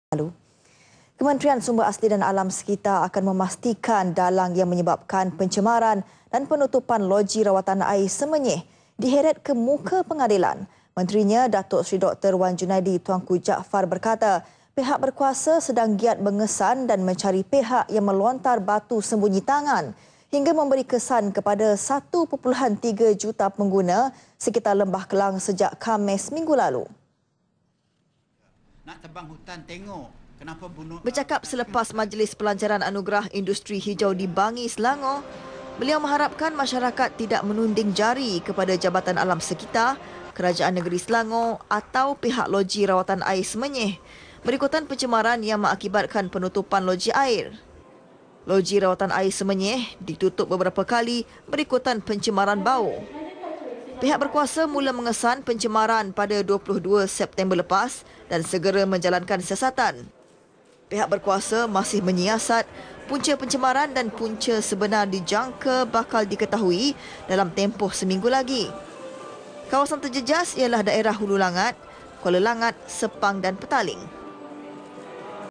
CARI DALANG LOJI RAWATAN AIR SEMENYIH TERCEMAR KE MUKA PENGADILAN - WAN JUNAIDI [1 OKT 2016]
Kementerian Sumber Asli dan Alam Sekitar akan memastikan dalang yang menyebabkan pencemaran dan penutupan Loji Rawatan Air Semenyih diheret ke muka pengadilan.

Menterinya Datuk Seri Dr Wan Junaidi Tuanku Jaafar berkata pihak berkuasa sedang giat mengesan dan mencari pihak yang "melontar batu sembunyi tangan" hingga memberi kesan kepada 1.3 juta pengguna sekitar Lembah Klang sejak Khamis minggu lalu.